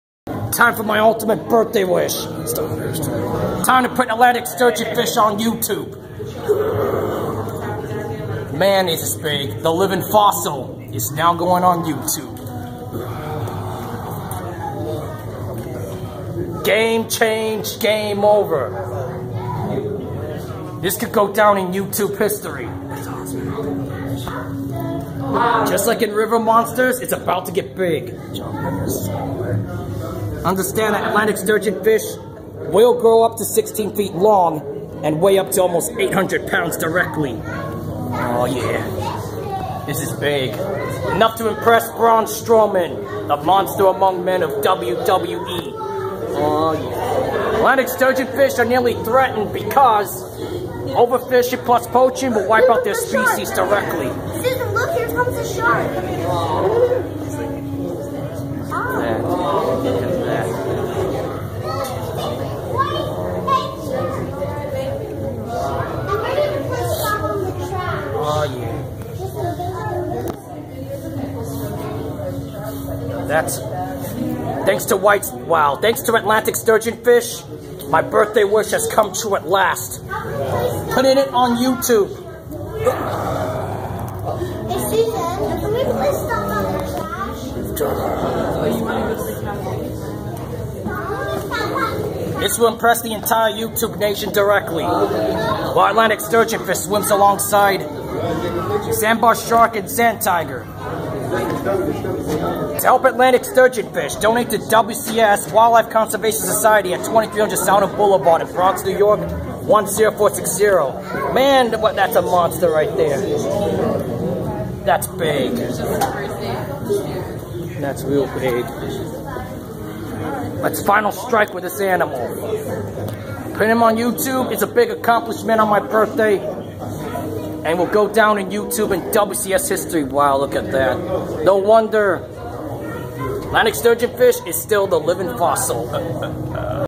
Atlantic Sturgeons at Ocean Wonders Sharks Exhibit
From Coney Island's New York Aquarium for its 1st visit throughout 2019 since my birthday on Friday, March 8, 2019. Facts about Atlantic Sturgeon:
1. Scientific Name = Acipenser Oxyrhynchus Oxyrhynchus.
2. A living fossil since the Mesozoic Era.
3. From Eastern North America🌎🇺🇸, New Brunswick in Canada🇨🇦, Western Europe🌍, & Western Africa🌍.
4. Nearly Threatened due to water pollution, overfishing, animal cruelty, habitat demolition, & POACHING.
5. Lives up to 60 years old just like African 🐘 & Asian 🐘.
6. Grows up to 16 feet long.
7. Biggest one weighs up to 800 pounds.
8. To help Atlantic Sturgeons, donate to Wildlife Conservation Society at 2300 Southern Blvd in Bronx, NY 10460.
9. Brown Tan or Bluish black but with a whitish belly.
10. Female Atlantic Sturgeon = Lays 800,000 to 3.75 million eggs in a single year for every 2-6 years.
11. Association of Zoos & Aquariums (AZA) teams up with WCS to increase & recover the Atlantic Sturgeon population.
12. Atlantic Sturgeon swims in brackish water from rivers to the Atlantic Ocean.
13. Longer snout with 4 barbels at the side of its mouth.
14. Five rows of bony plates called scutes.
15. Tail like the shark's where one side called a lobe is larger than the other.
16. Eats 🦀, mussels, & insects like worms on the bottom.
17. Spends its first years of its life in East New Jersey & West New York on the Hudson River & then swims out towards the Atlantic Ocean.
18. Nickname = Atlantic Sturgeonfish.
Spoiler = The Atlantic Sturgeons from Ocean Wonders 🦈 exhibit at Coney Island's NY Aquarium in Brooklyn were moved to the Sea 🐢 tank since Friday, 5/27/2022 during my Cousin Werner's visit to NYC from Friday, 5/20/2022 to Sunday, 5/29/2022 before Memorial Day since Monday, 5/30/2022 & become an Atlantic Sturgeon tank outside & nextdoor to PlayQuarium & Spineless Exhibits at Coney Island's NY Aquarium.